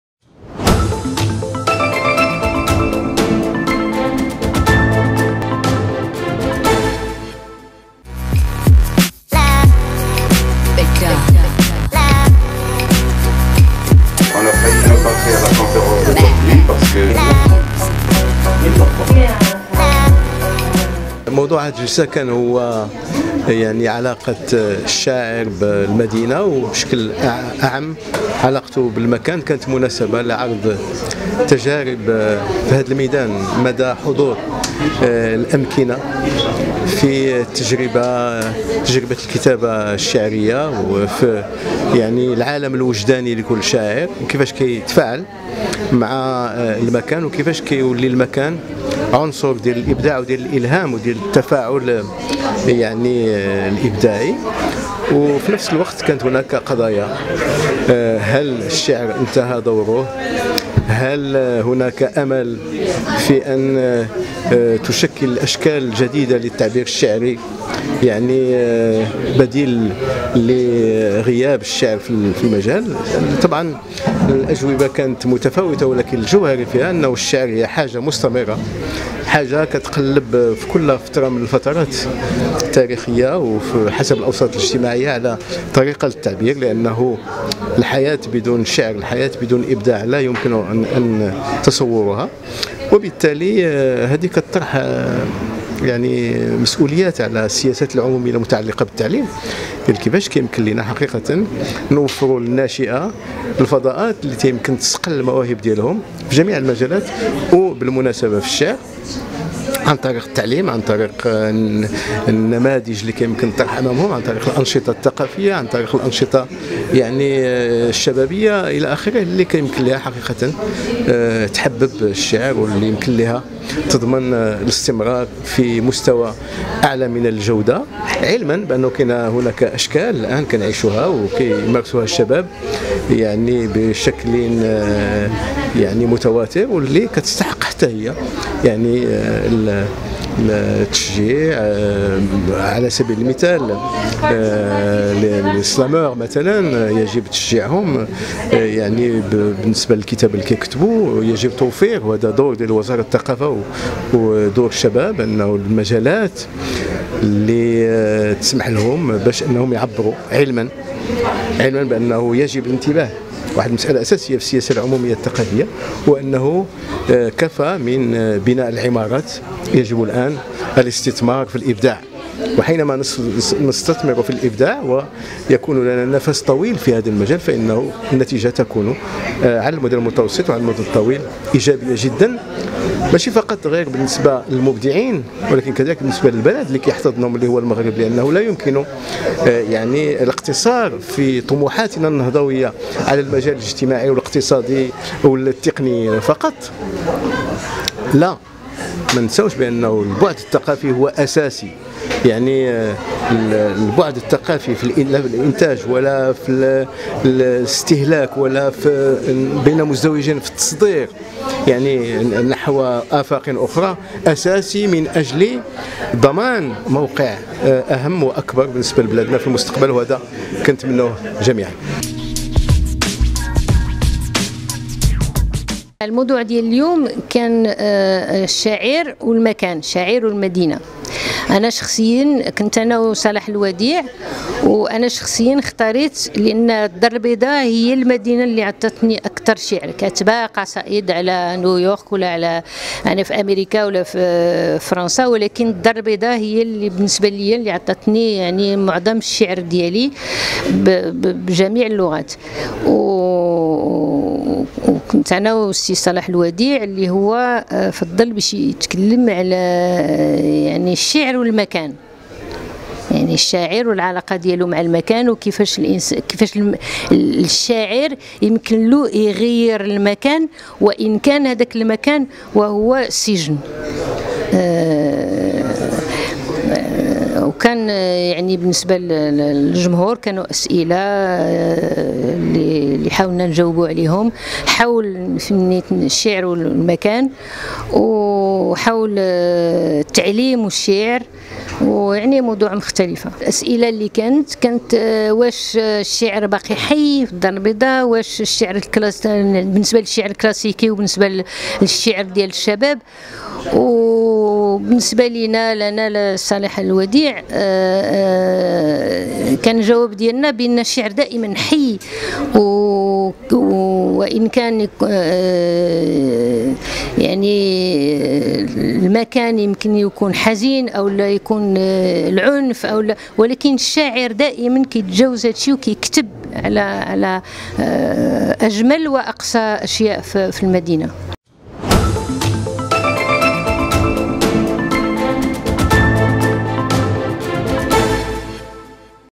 الشاعران صلاح الوديع وحفصة لمراني ينبشان في علاقة الشعر بالمكان
# أخبار_عاجلة